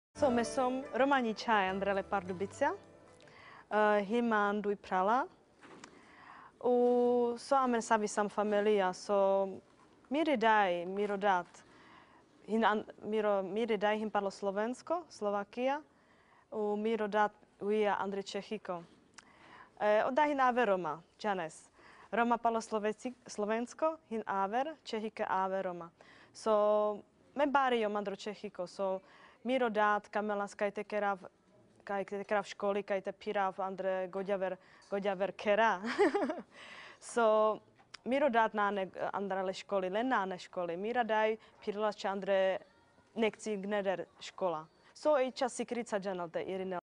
Lucie Horvathova
March 2011 Lucie speaks about her family and Romani language.